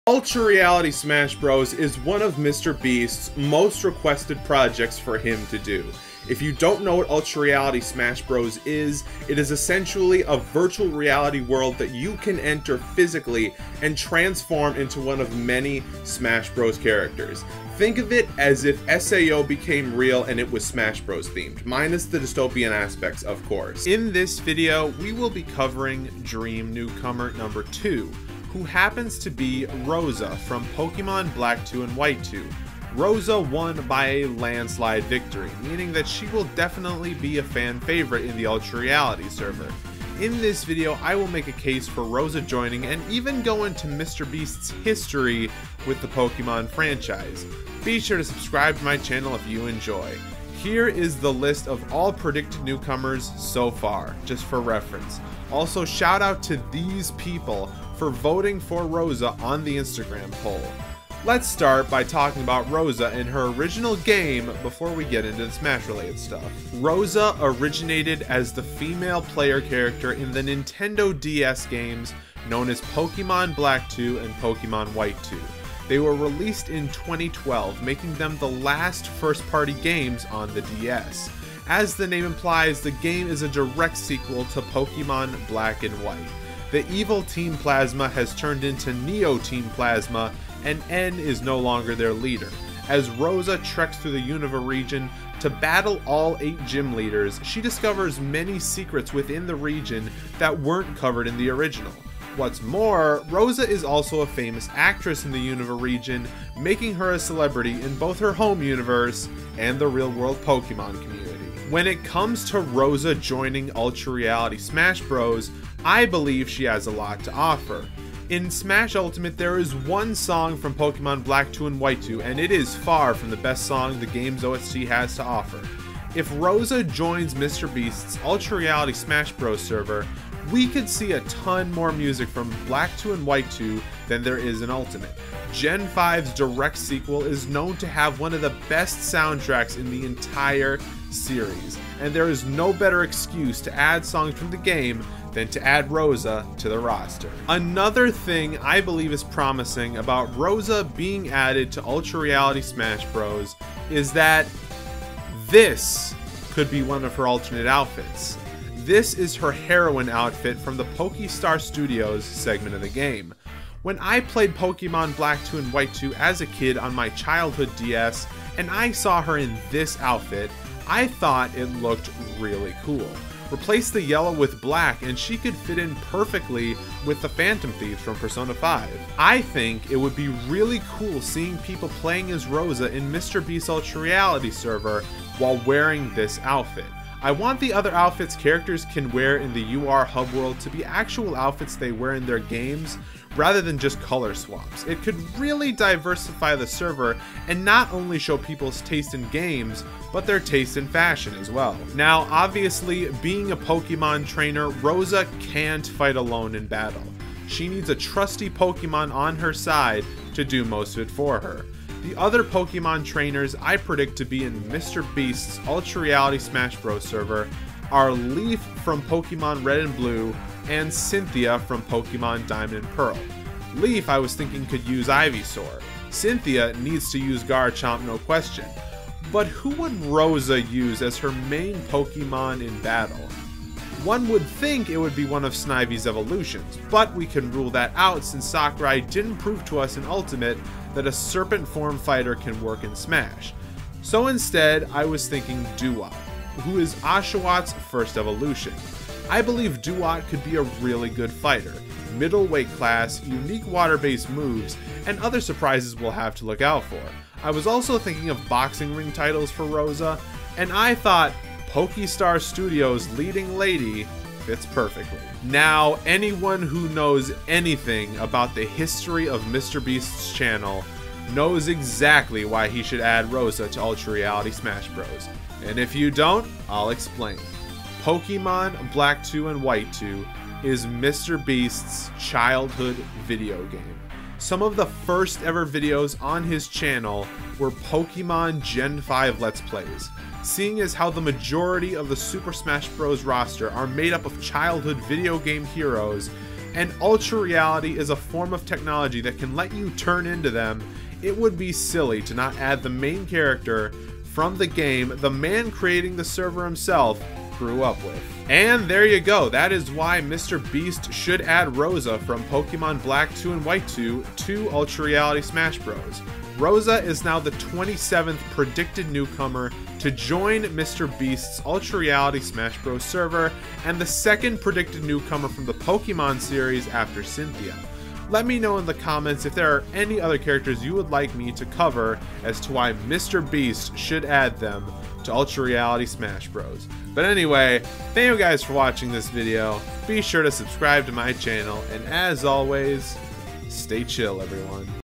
MrBeast ULTRA REALITY SMASH BROS Dream Newcomer #2: Rosa (Pokemon Black 2/White 2)
Not only did Rosa win the Instagram poll by a landslide victory, but she also is the protagonist of MrBeast's childhood game. Rosa is a must in Ultra Reality Smash Bros so I genuinely needed to talk about why she must join!